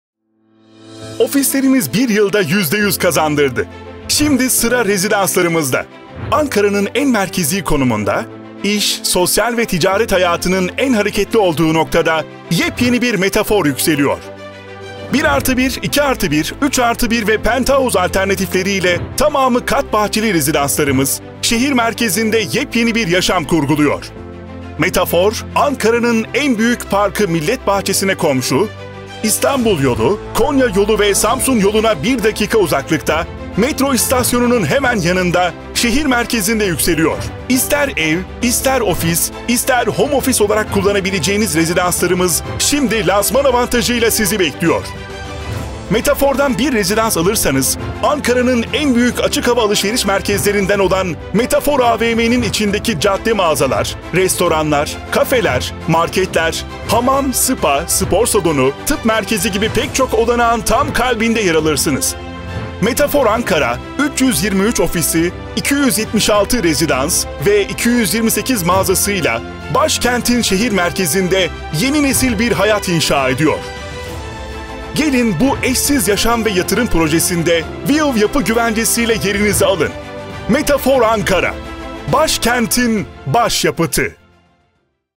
Metafor Ankara Rezidans Bloğu Tanıtım Videosu
Ofislerimiz 1 yılda %100 kazandırdı, şimdi sıra rezidanslarımızda!

Ankara’nın en merkezi konumunda, iş, sosyal ve ticaret hayatının en hareketli olduğu noktada, yepyeni bir metafor yükseliyor.

1+1, 2+1, 3+1 ve penthouse alternatifleriyle, tamamı kat bahçeli rezidanslarımız şehir merkezinde yepyeni bir yaşam kurguluyor.

Metafor, Ankara’nın en büyük parkı millet bahçesine komşu, İstanbul Yolu, Konya Yolu ve Samsun yoluna 1 dakika uzaklıkta, metro istasyonun hemen yanında şehrin merkezinde yükseliyor.

İster ev, ister ofis, ister home ofis olarak kullanabileceğiniz rezidanslarımız şimdi lansman avantajıyla sizi bekliyor!

Metafor’dan bir rezidans alırsanız, Ankara’nın en büyük açık hava alışveriş merkezlerinden olan Metafor AVM’nin içindeki cadde mağazalar, restoranlar, cafeler, marketler, hamam-spa, spor salonu, tıp merkezi gibi pek çok olanağın tam kalbinde yer alırsınız.

Metafor Ankara 323 ofisi, 276 rezidans ve 228 mağazası ile Başkentin şehir merkezinde yeni nesil bir hayat inşa ediyor.

Gelin bu eşsiz yaşam ve yatırım projesinde View Yapı güvencesi ile yerinizi alın.